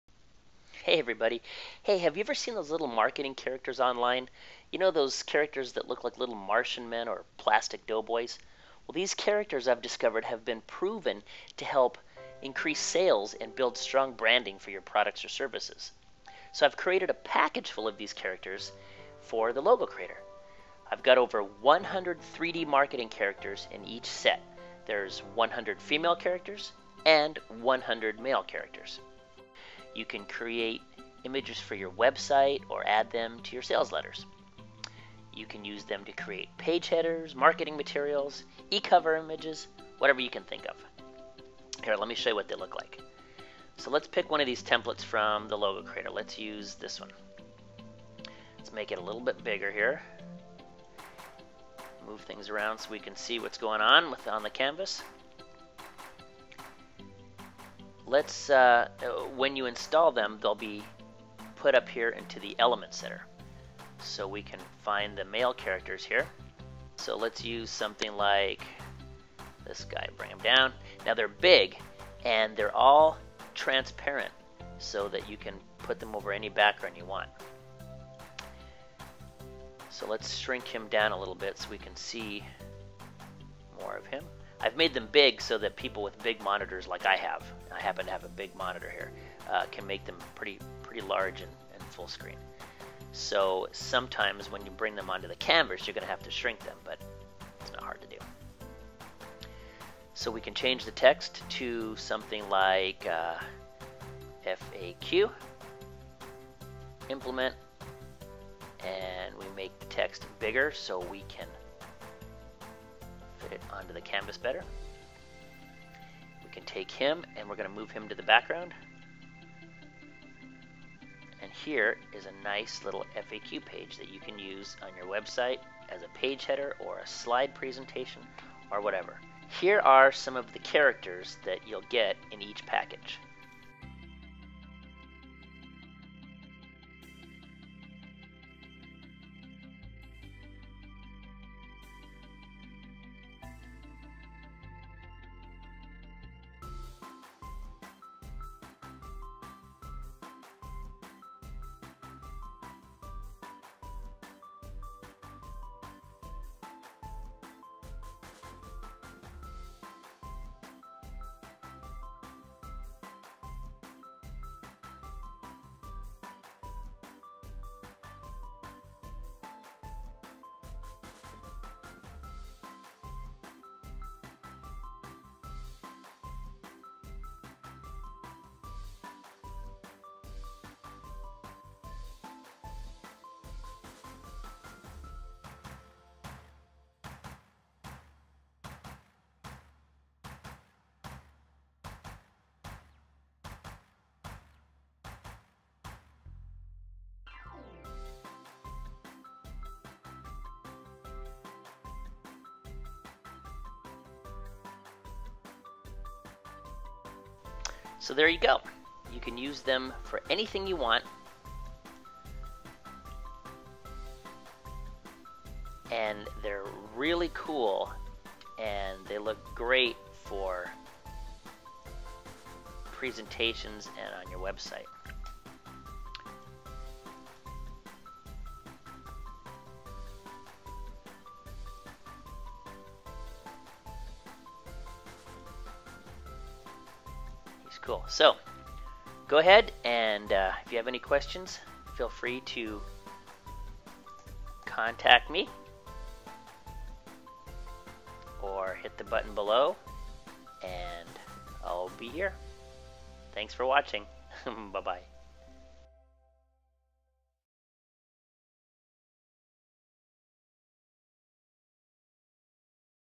3D characters for The Logo Creator
Check out these awesome 3D Marketing characters available for The Logo Creator software version 6.0!

There are 100 3D Male Characters and 100 3D Female Characters! Use them for page headers, marketing material, your website,blogs and social network groups!